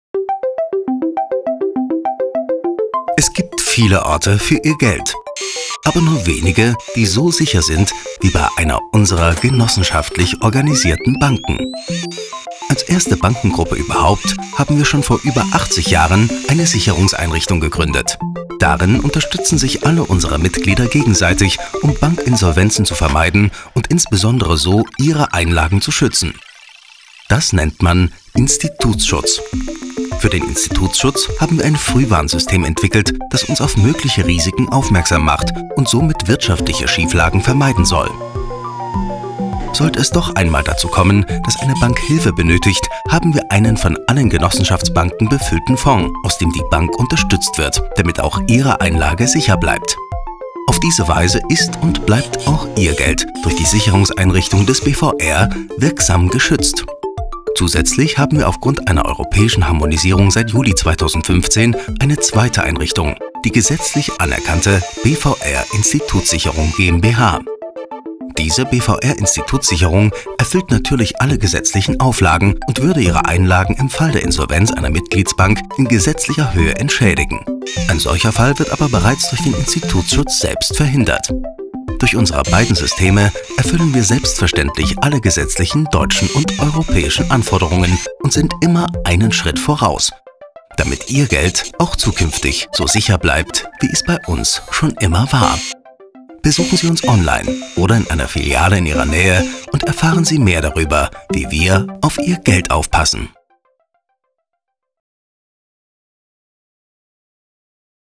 Einlagensicherung: Verständlich erklärt
Ihre Volksbank Mittelhessen ist der amtlich anerkannten BVR Institutssicherung GmbH und der zusätzlichen freiwilligen Sicherungseinrichtung des Bundesverbandes der Deutschen Volksbanken und Raiffeisenbanken (BVR) angeschlossen. Gemeinsam gewährleisten diese den Schutz Ihrer Einlagen.